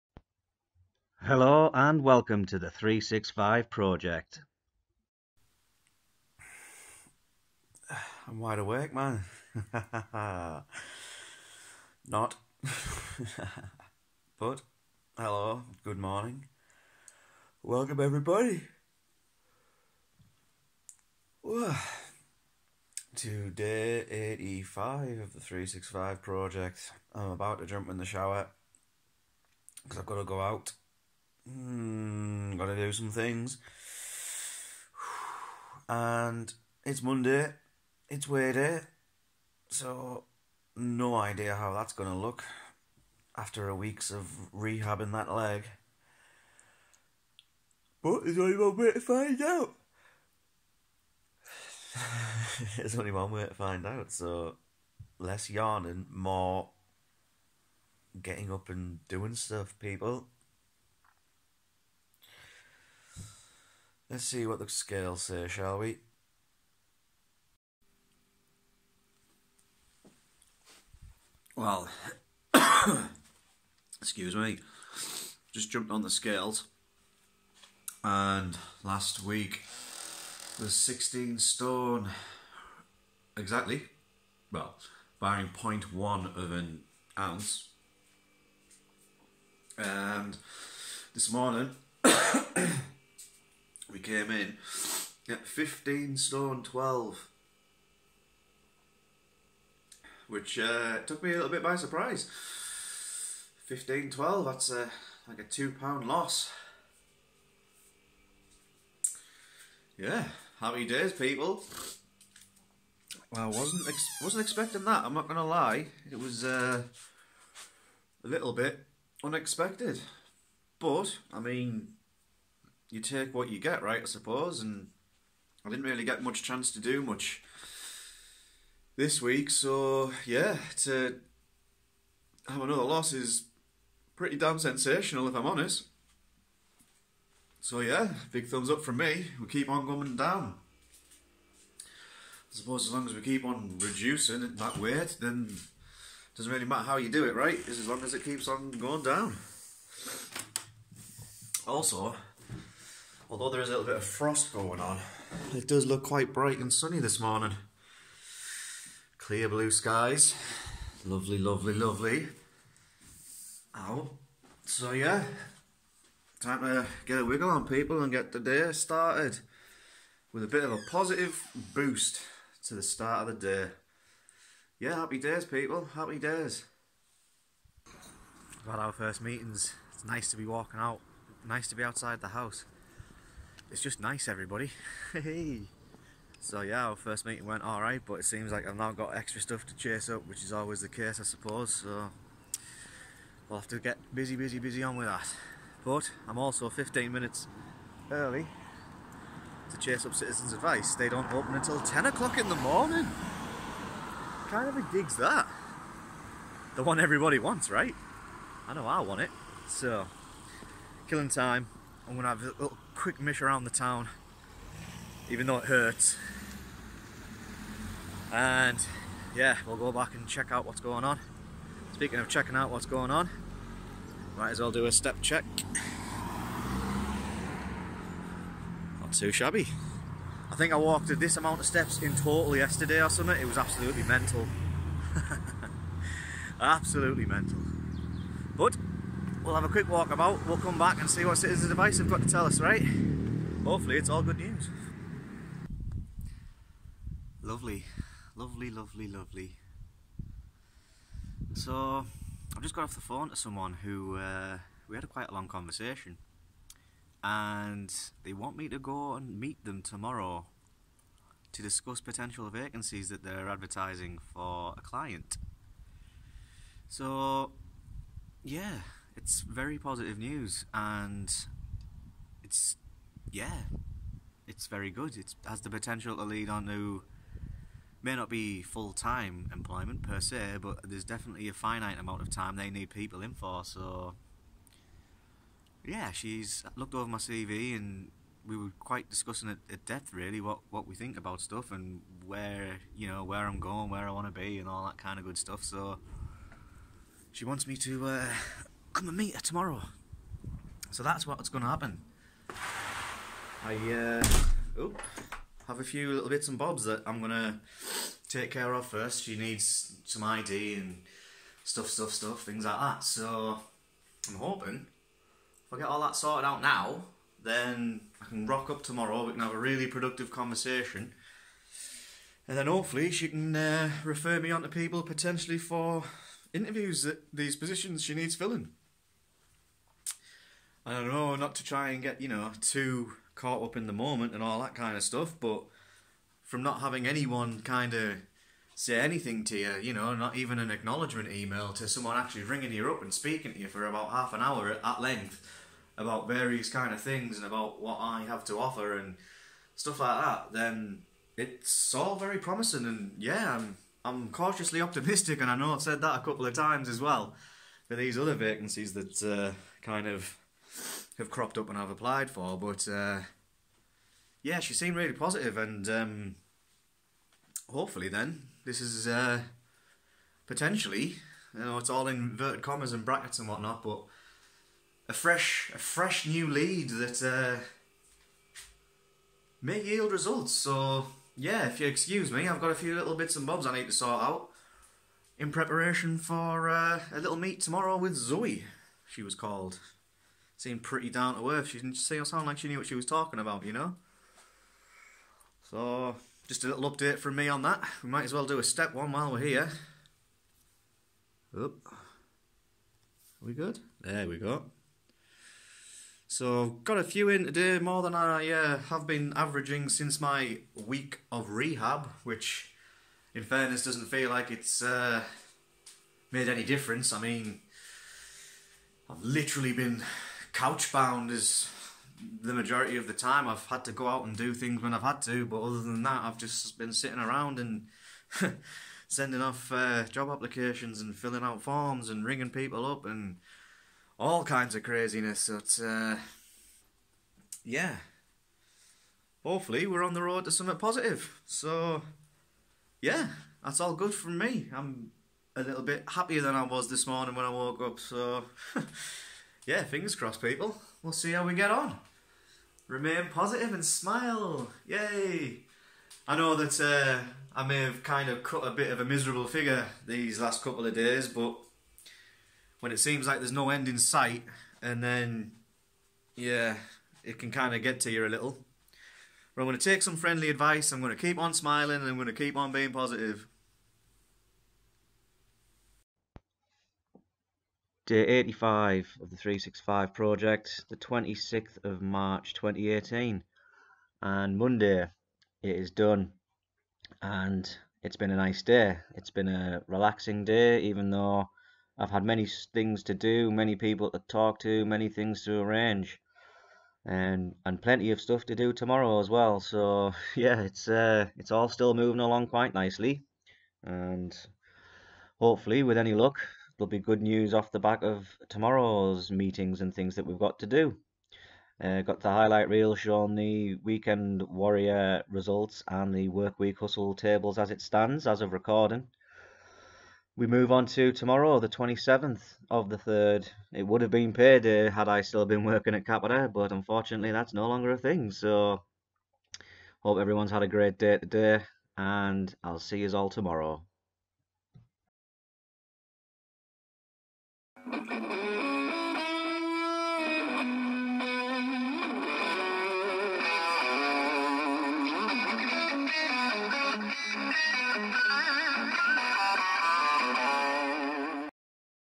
365 Project 26 03 18
Day 85 - First meeting with Jobcentre today, lets get the ball rolling on getting a new job.